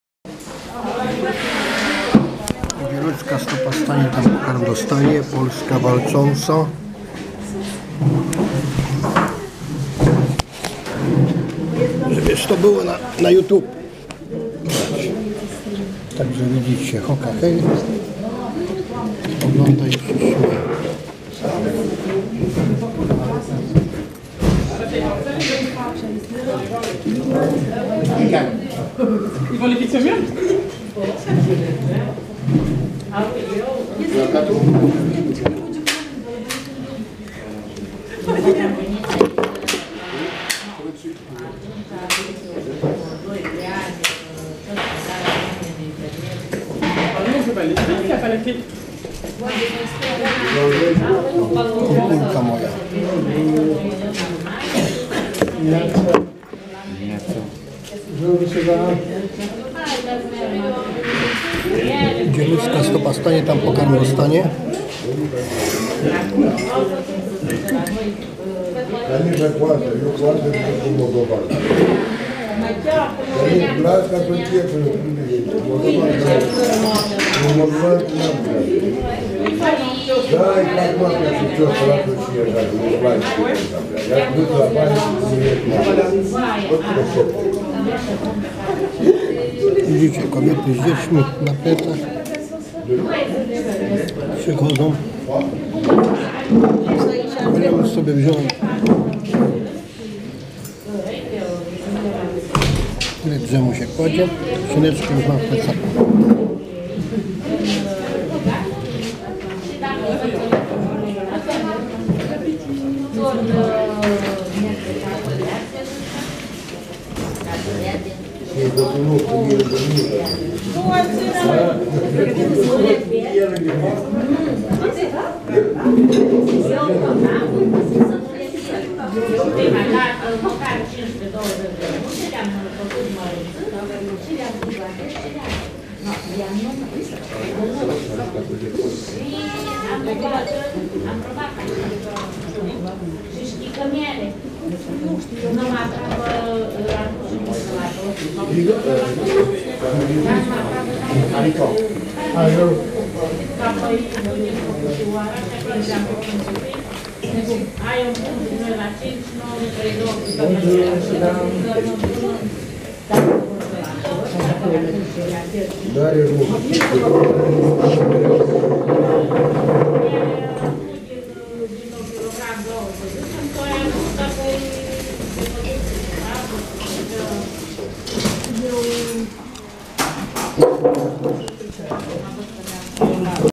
MVI 6121 Dzisiejsze Sniadanie w Chenay-Gagny w Boutique Solidarite w Fundacji Abe Pierre siewidzialo
Gdzie Ludzka stopa stanie tam pokarm dostanie!Ja dostalem ze az sie dzis przejadlem!HOWGH!I wykpaie sie zaraz!